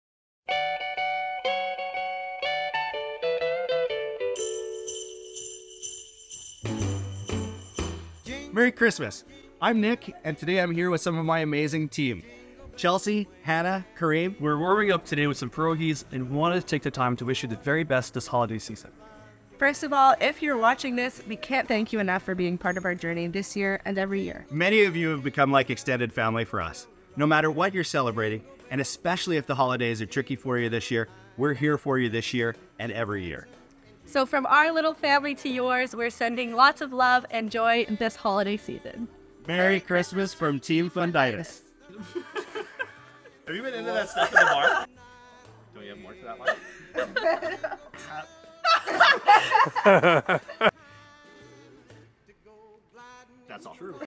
Happy Holidays from Team Nick Fundytus
From our hearts to your screens, we hope this message finds you well.
You mean a lot to us, and we wanted to take some time to remind you of that.

Warmest wishes,
Team Nick Fundytus:
Nick, Karim, Chelsea, Hannah, and Jan behind the camera.